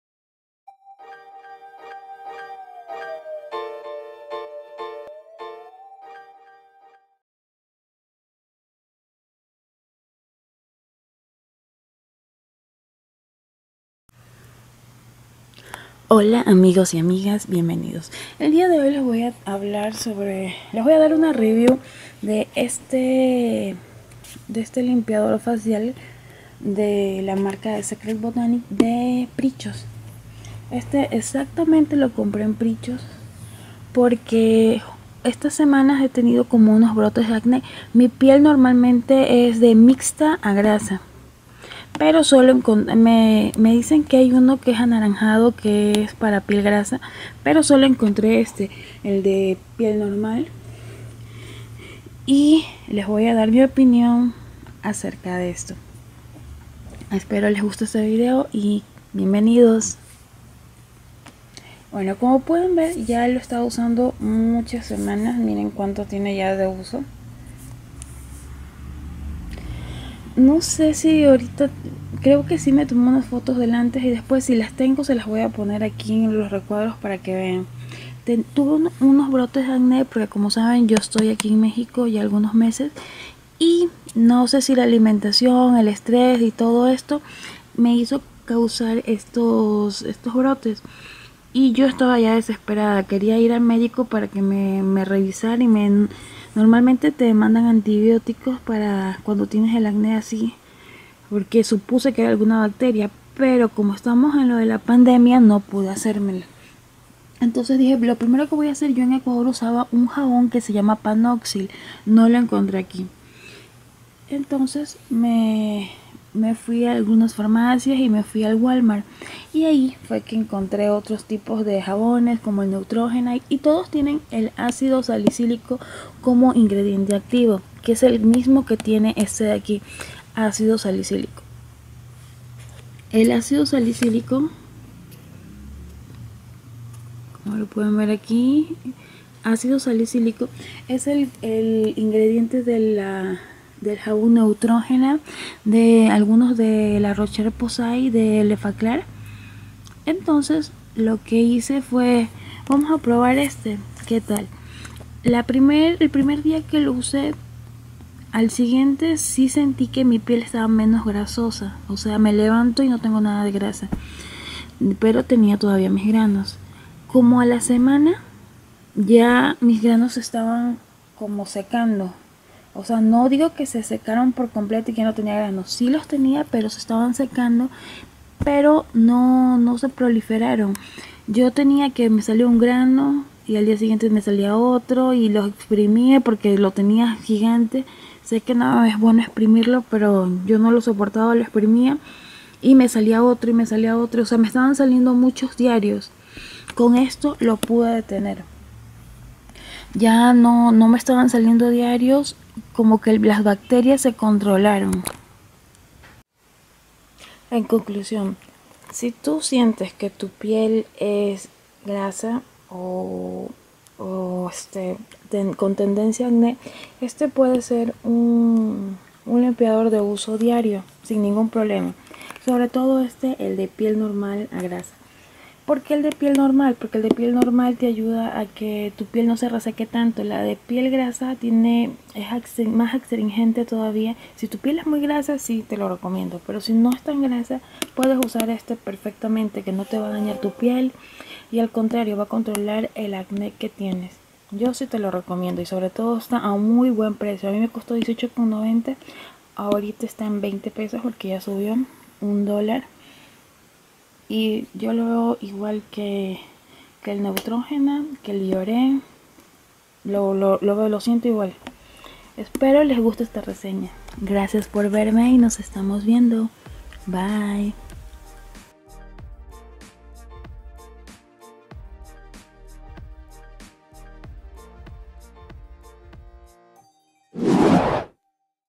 Review jabon oil free acne wash para el acné comprado en prichos
Hola amigos y amigas, aqui les traigo mis experiencia usando este jabón espero les guste y les sirva, gracias